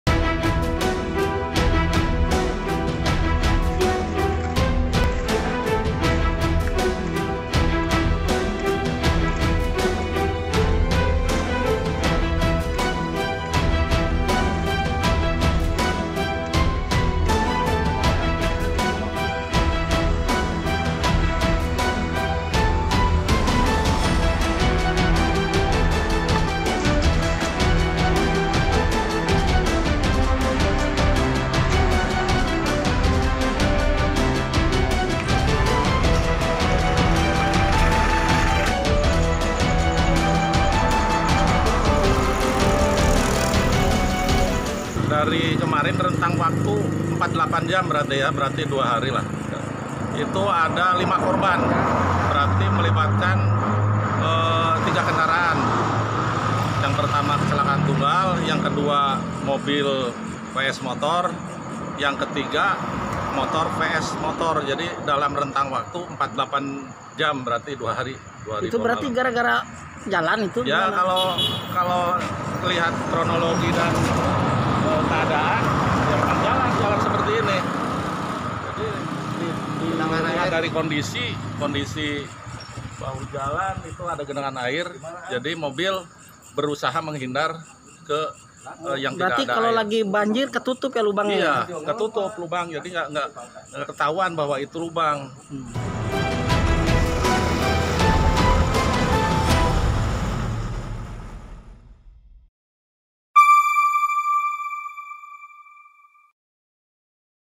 Jalan Rusak Parah Picu Kecelakaan di Pangandaran
Inilah video amatir yang merekam beberapa kejadian kecelakaan akibat jalan rusak. Kurang dari 48 jam, telah terjadi 3 kecelakaan akibat jalan rusak itu.

Android: inews.id/android
iPhone: inews.id/ios